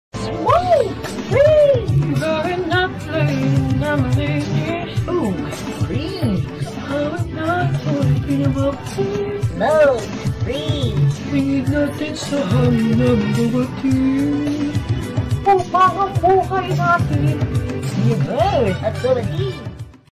JAYA || IMPERSONATES Smoke Free (with Goofy, Spongebob, Jessa/Toni Gonzaga) Voice Over
Enjoy watching KaFresh Berginnn